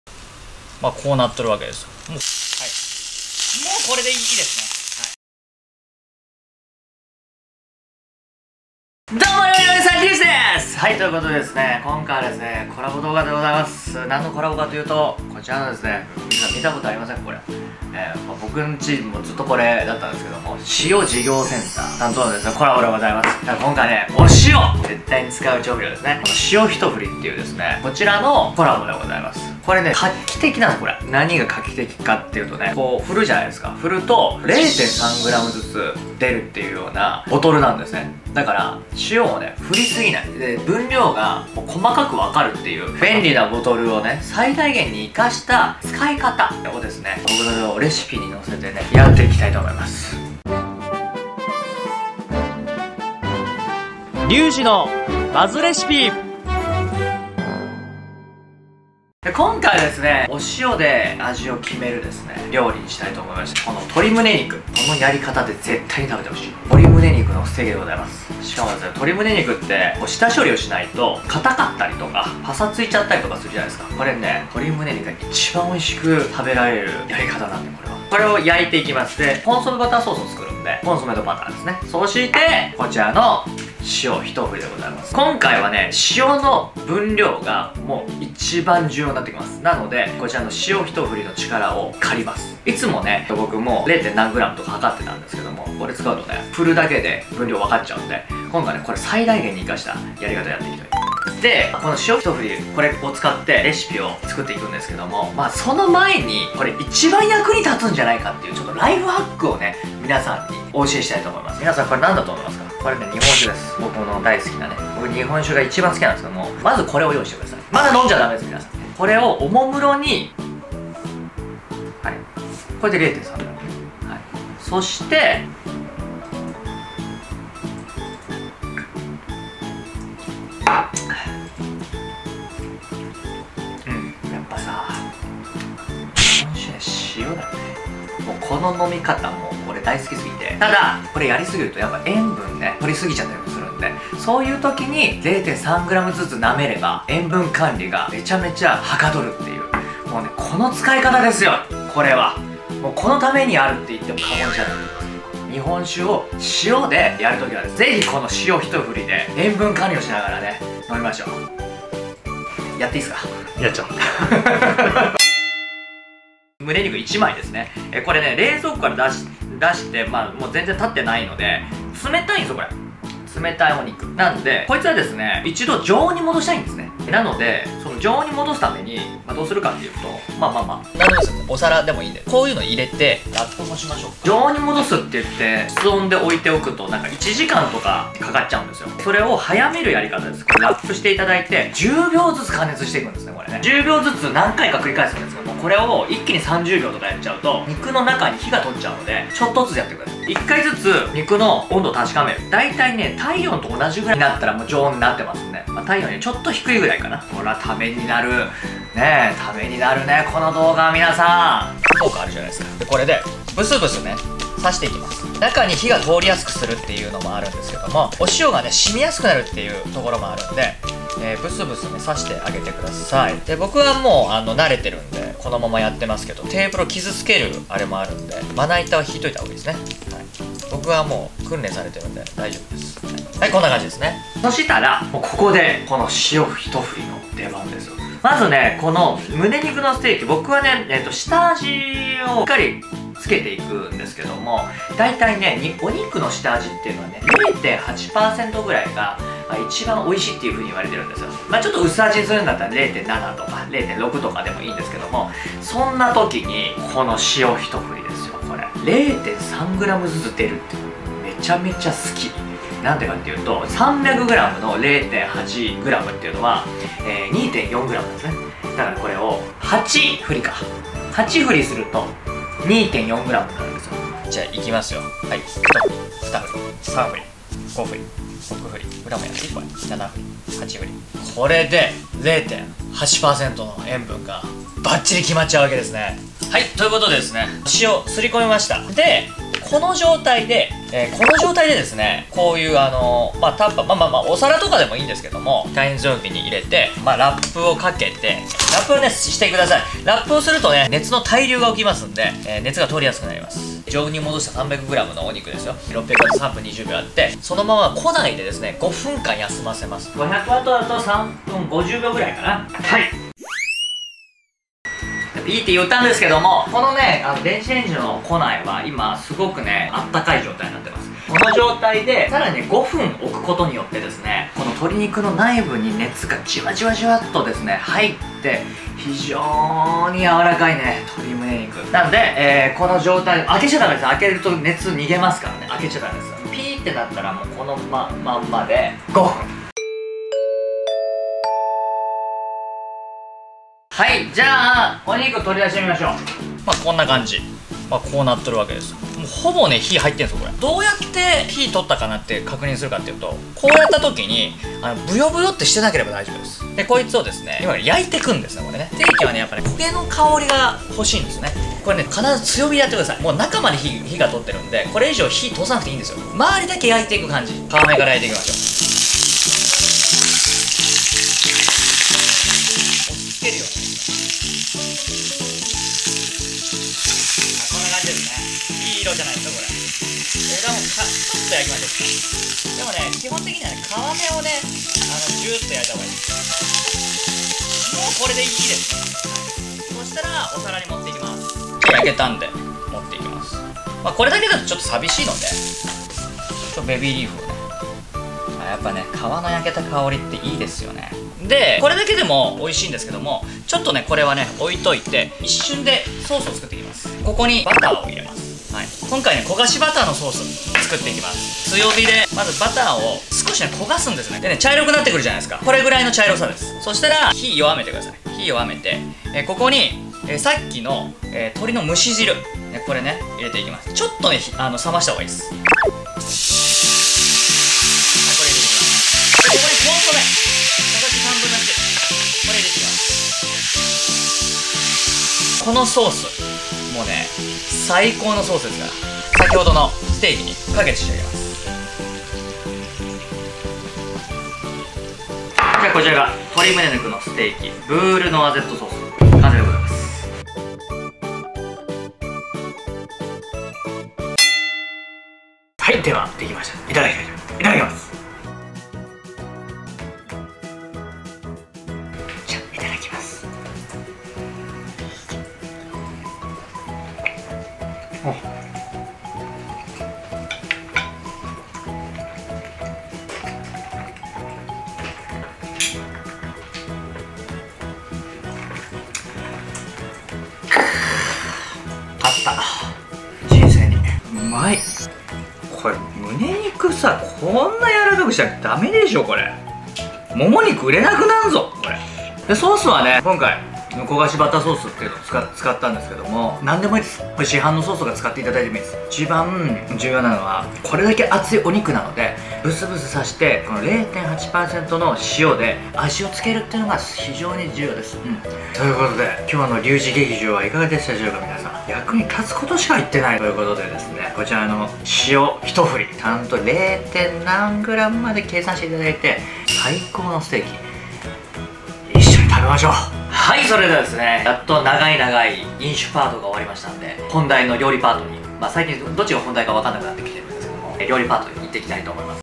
100g58円の鶏胸肉がヤバい柔らかくなる、史上最強にウマい筋肉痩せ飯【極厚鶏胸肉ステーキ】
【鶏胸肉のステーキ】

公益財団法人塩事業センター様とのコラボでした！

★今回のレシピはこちら↓
鶏胸肉300g
塩　2.4ｇ
（【塩ひとふり】で８振りになります）
サラダ油　大さじ１
バター15g
コンソメ小さじ1/3

胸肉はレンチンしてから焼くと丸ごとでも驚く程柔らかなステーキに！
鶏胸肉300gは常温に戻しフォークで全体を刺し塩小さじ1/3を擦り込み600w3分20秒チンし5分放置油で両面を50秒ずつほど焼き取り出しバター15gを焦がして鶏の蒸し汁とコンソメ小さじ1/3入れソースにし完成！

フリーBGM
DOVA-SYNDROME